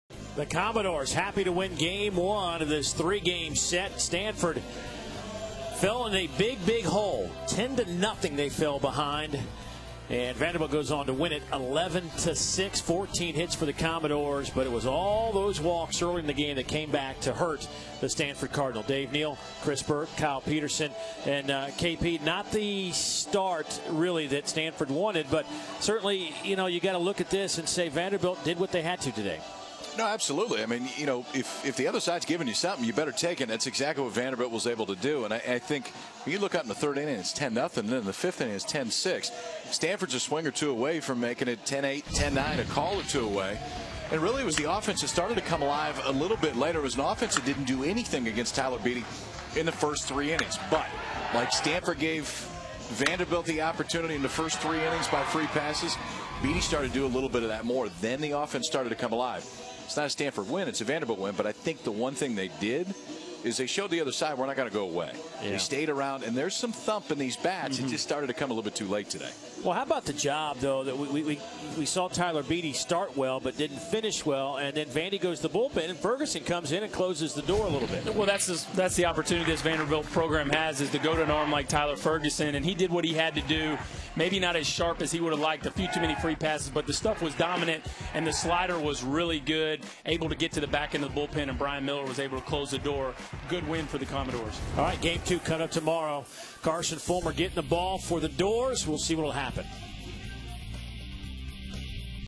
Vanderbilt Tops Stanford In Series Opener
A recap of Vanderbilt's 11-6 victory over Stanford in game one of the super regional college baseball playoffs.